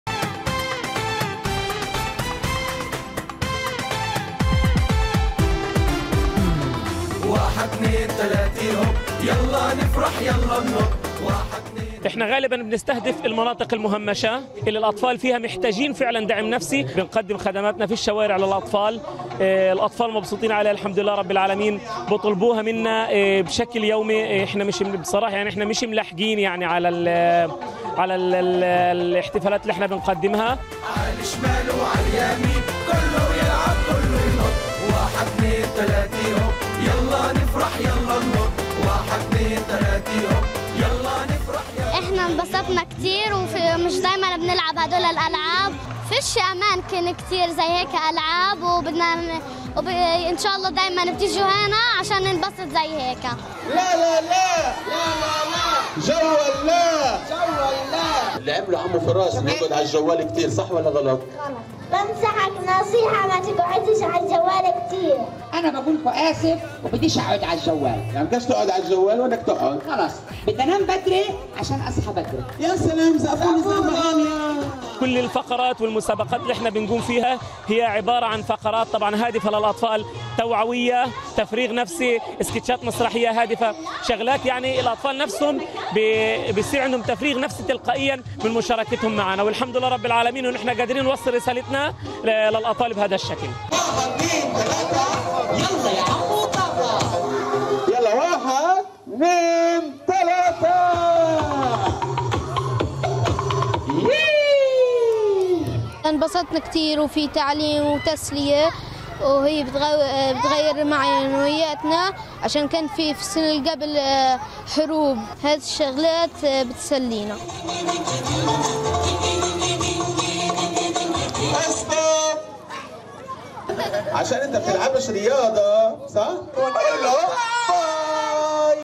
فريق أيام الفرح في غزة يقدم عروضاً مسرحية ترفيهية وتوعوية للأطفال
وسط حلقة كبيرة من الأطفال يقف فريق أيام الفرح في غزة ليقدم عروضاً مسرحية ترفيهية وتوعوية، إضافة إلى اللعب والمشاركة في نشاطات تُدخل البهجة إلى قلوب أطفال القطاع المحاصر. مبادرة لاقت إقبالاً من الأطفال وخصوصاً أولئك الذين يعانون أوضاعاً صعبة نتيجة الحروب الإسرائيلية.

إعداد: وسام العشي